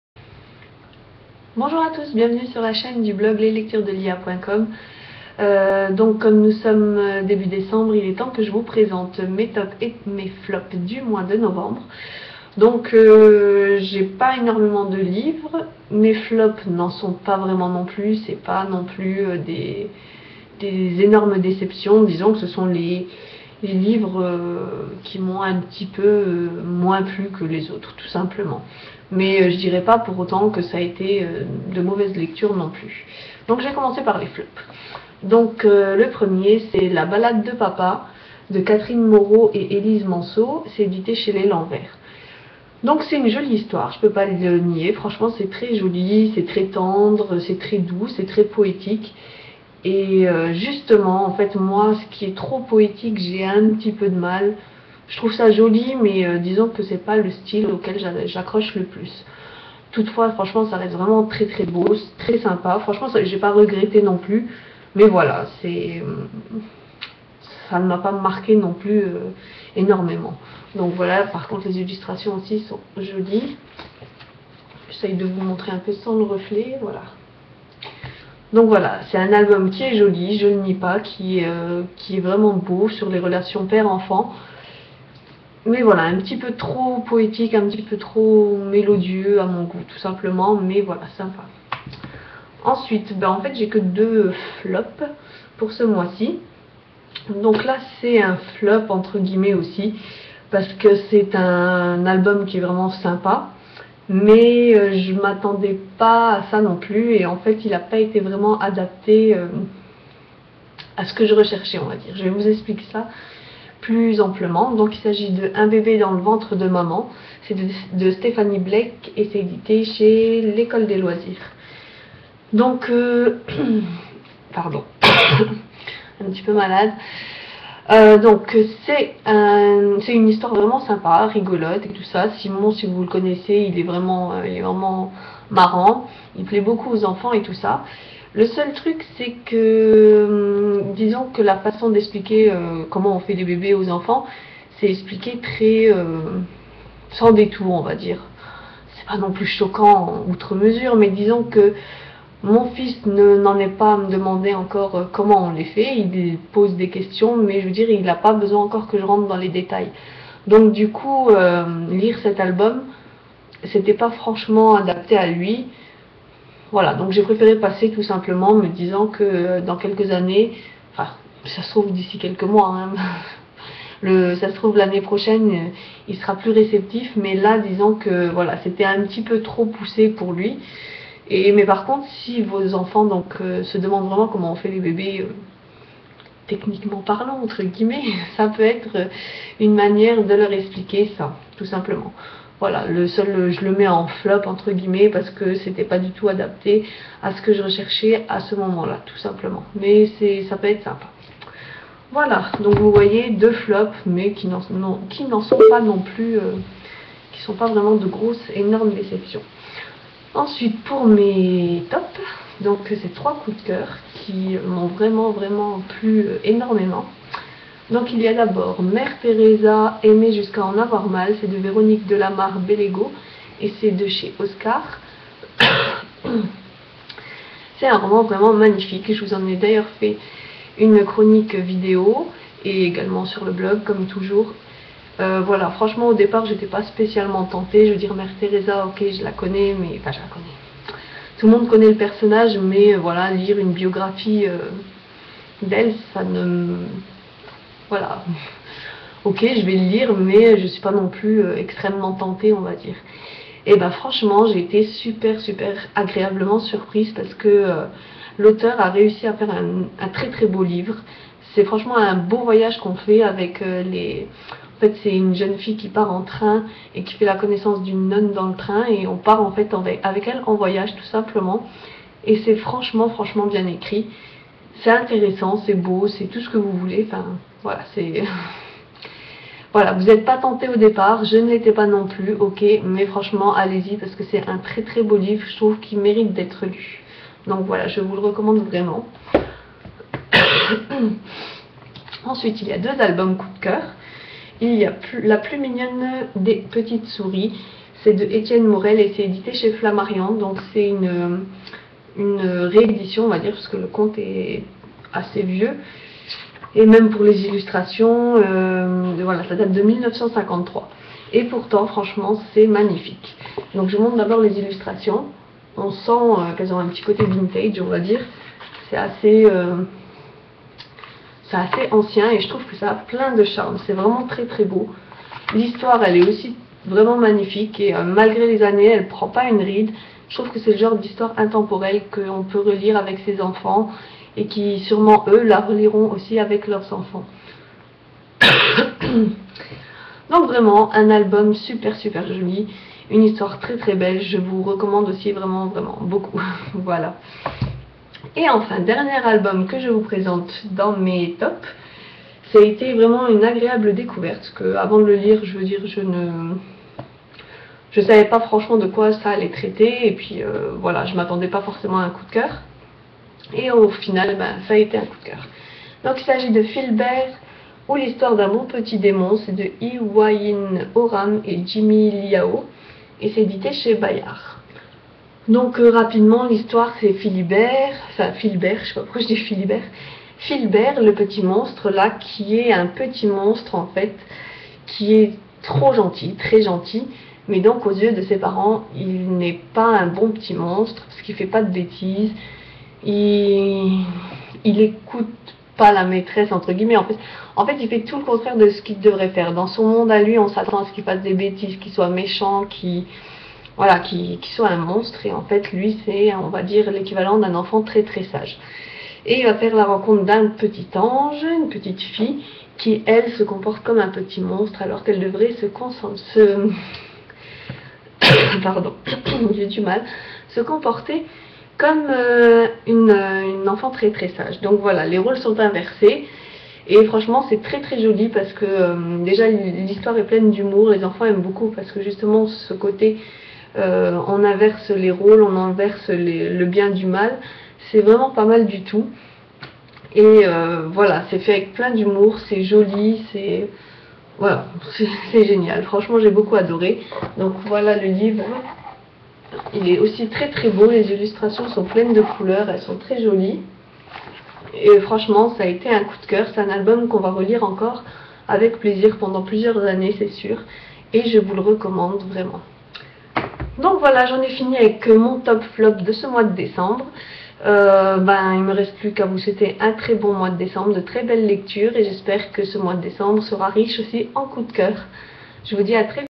Top / Flop - Novembre 2013
Voici mon TOP / FLOP de novembre 2013 !

2 Flop pour 3 Top ce mois-ci !